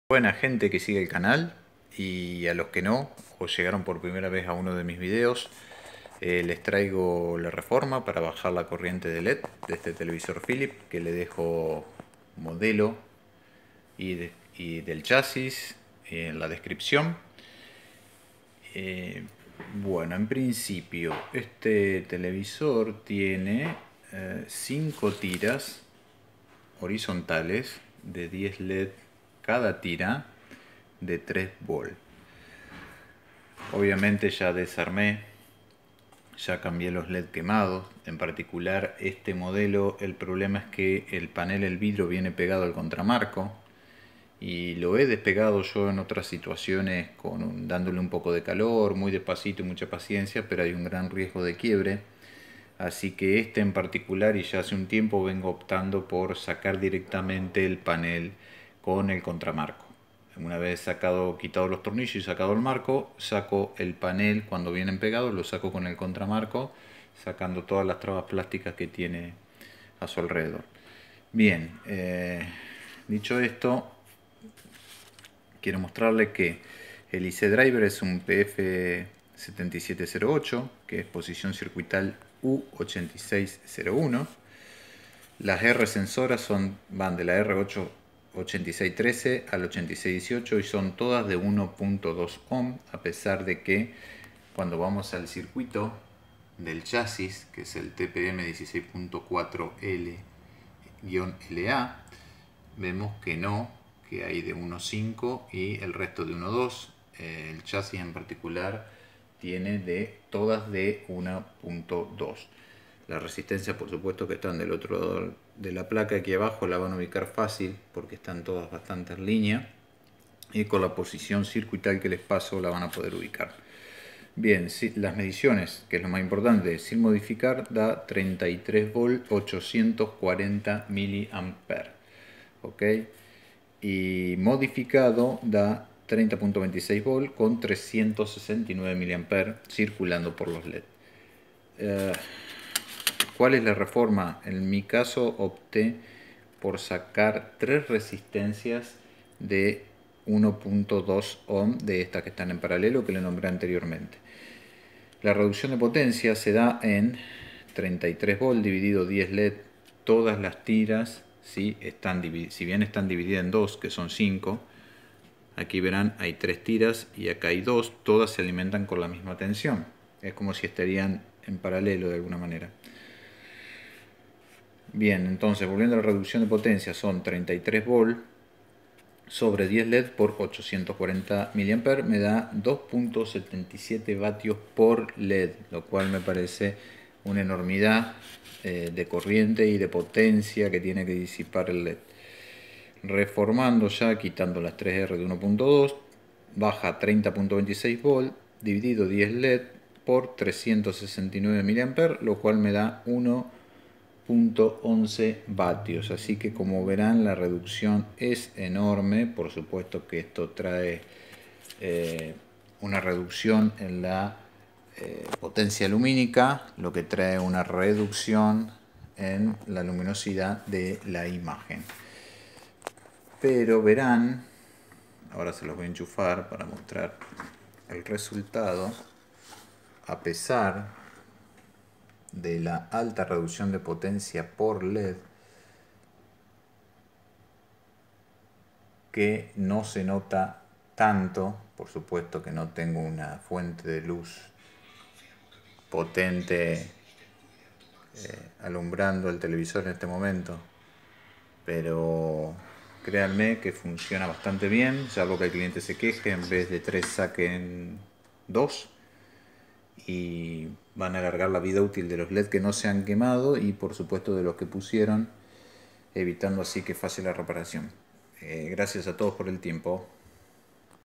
43pfg5101/77, tpm16.4l la, pf7708bs, r8613 ,8618, reducir potencia de leds, pin 8 fb de ic driver
SI LES FUE UTIL, LES DEJO UN LINK PARA QUE ME INVITEN UN CAFECITO, GRACIAS.

buenas gente, les comparto el metodo de baja de potencia de leds de back light, ver que en este caso las resistencia a modificar van al pin 8 del ic driver pf7708, que es  fb (feedback), y no a cs (current sense) pin 3 del ic ( que también es ovp, over voltaje protection) como en otros ic y fuentes.